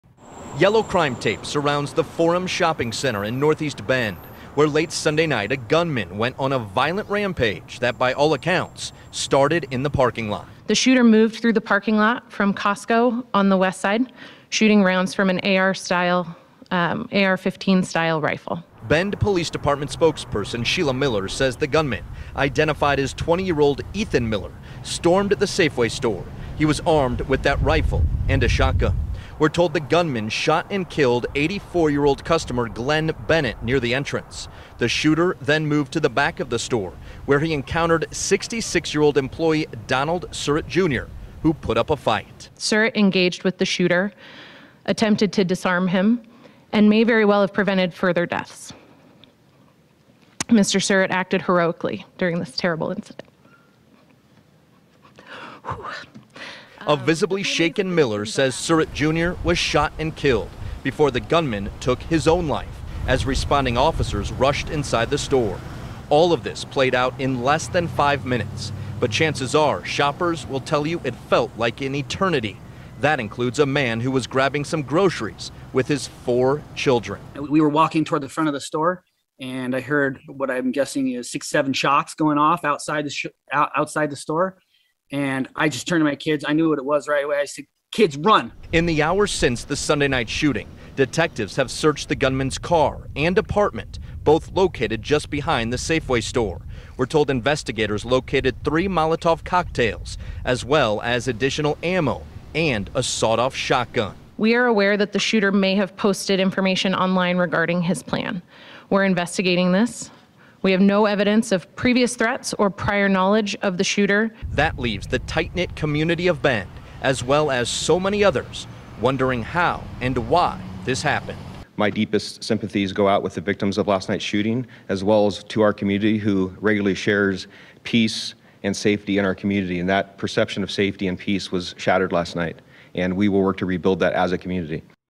Deadly shooting at a Safeway in Bend ended with three people dead, including the gunman
Investigators said it could've been worse than it was if not for the heroic actions of a Safeway employee.